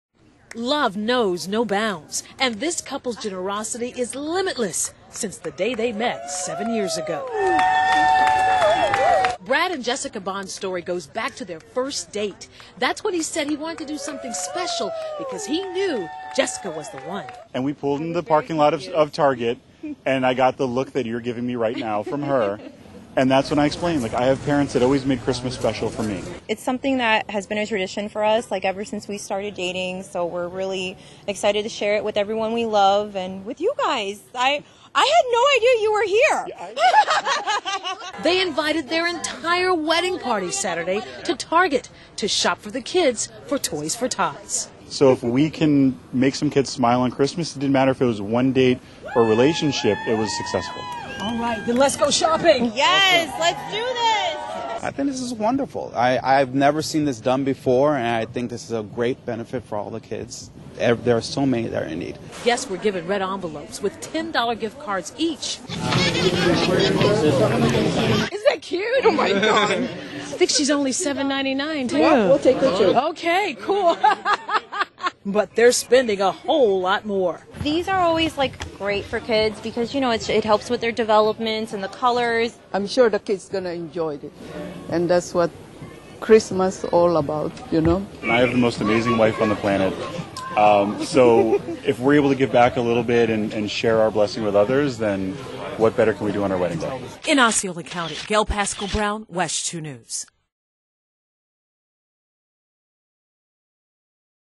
Florida bride and groom take wedding guests to Target to shop for kids in need
Florida bride and groom take wedding guests to Target to shop for kids in need (WESH)